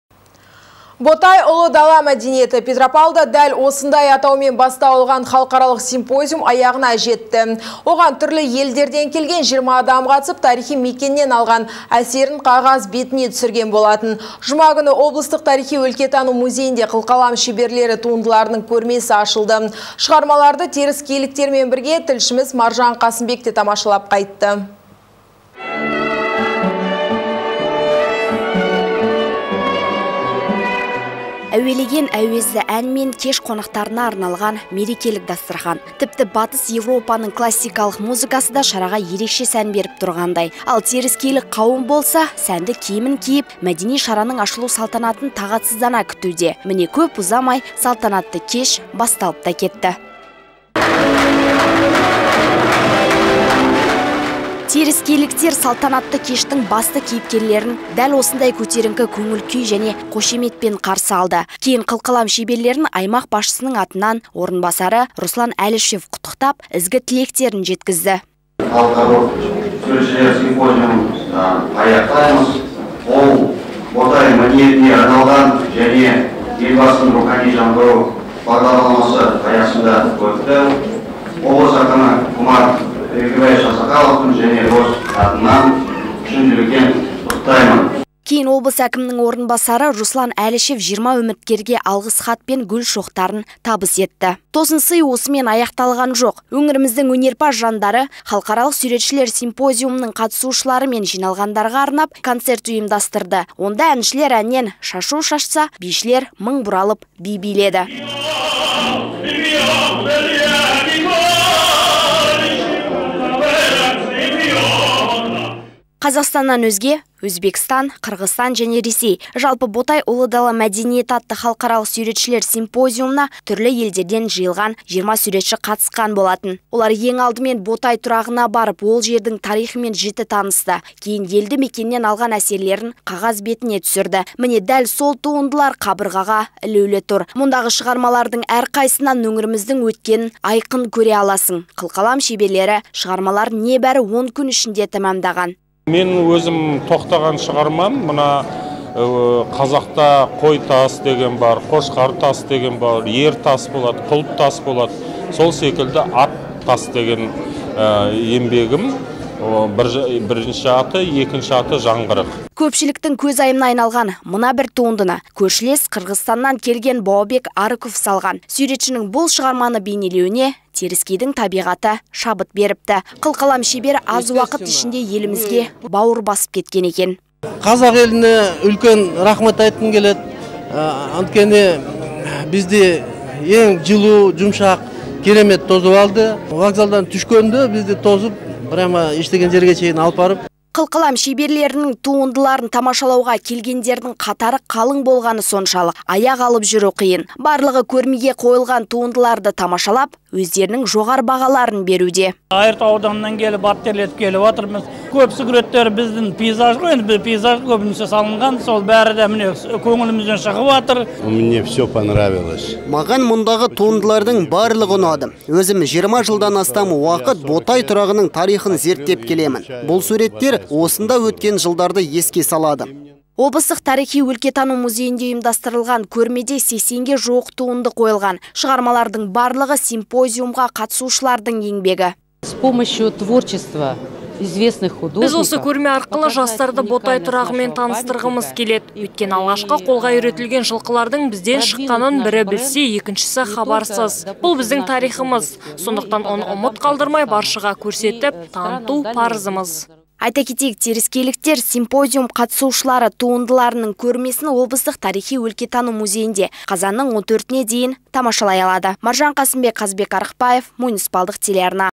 "Ботай - ұлы дала мәдениеті” атты көрме қанат жайды
"Ботай - ұлы дала мәдениеті”. Петропавлда дәл осындай атаумен бастау алған халықаралық симпозиум аяғына жетті. Оған түрлі елдерден келген 20 адам қатысып, тарихи мекеннен алған әсерін қағаз бетіне түсірген болатын. Жұма күні облыстық тарихи-өлкетану музейінде қылқалам шеберлері туындыларының көрмесі ашылды. Шығармаларды теріскейліктер мен бірге тілшіміз Маржан Қасымбек те тамашалап қайтты.